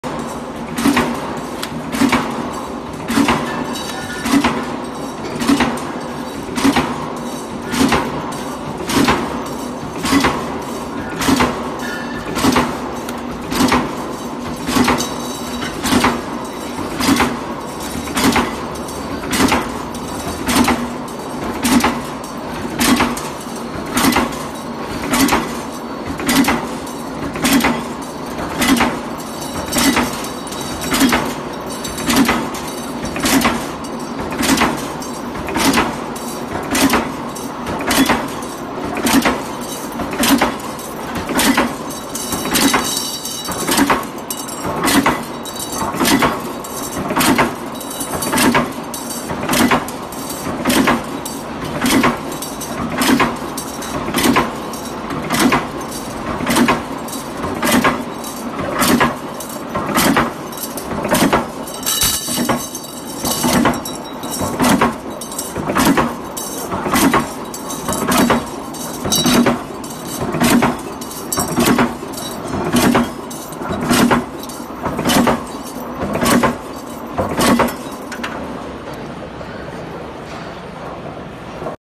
MYT® brand USA type TDF / TDC duct corner making machine automatic TDF corner punching line
J23-63T automatic TDF corner/TDC corner punching line with CNC control . Working perfectly for American type . And TDF/TDC mould are finish one time . Have high efficiency for clients .